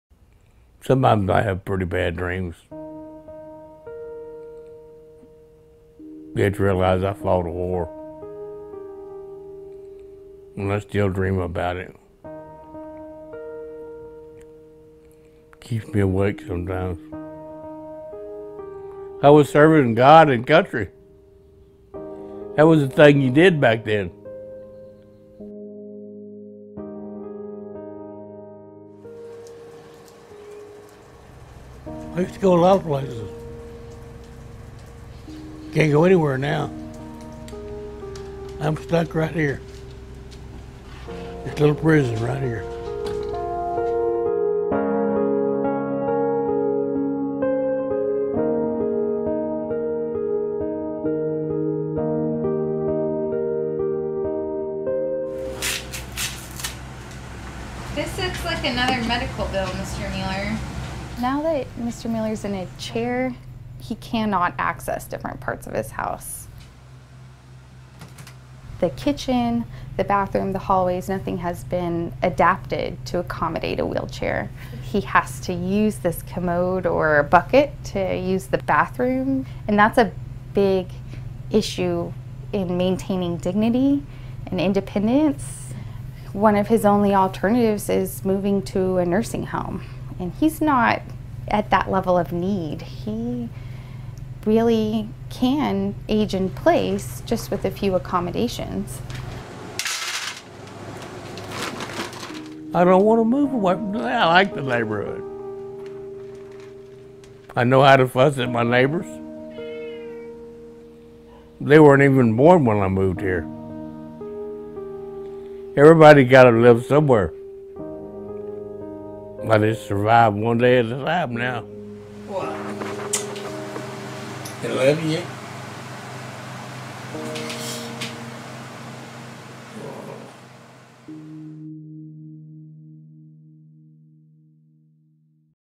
Season for Caring 2015: Gordon Meeler
Gordon Meeler has lived in his south Austin trailer for the last 29 years, but his recent mobility issues now make it impossible for him to get to the bedroom or bathroom of his house. He's trapped in his living room, what he calls a “little prison."